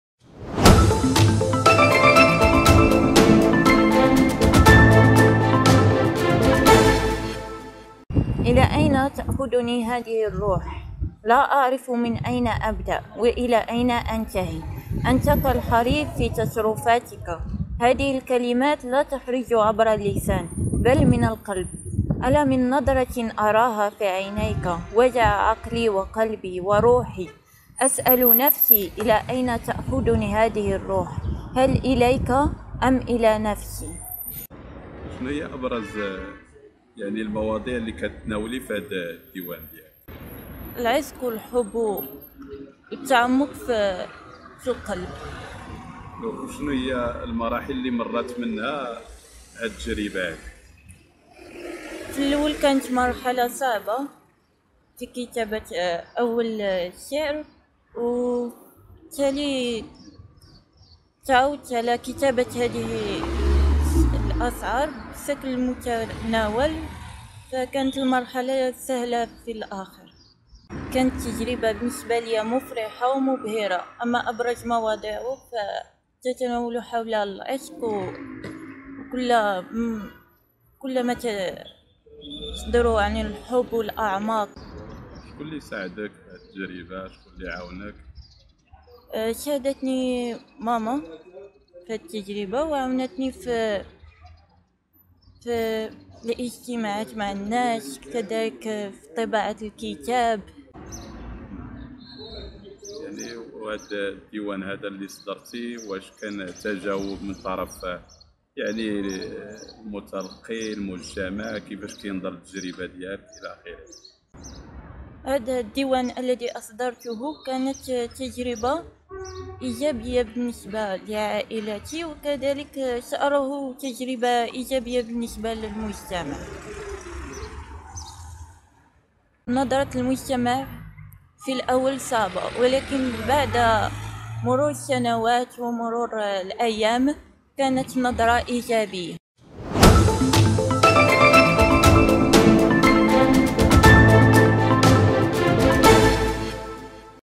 الشابة خديجة بلعربي توقع بفاس كتابها " مقامات العشق "
يتطرق الفيديو الى الديوان الجديد الذي أصدرته خديجة بلعربي، كما يستعرض أبرز المواضيع التي تتطرق لها الشاعرة الشابة، والى الصعوبات التي واجهتها وهي تقرر خوض غمار الكتابة

# أخبار_عاجلة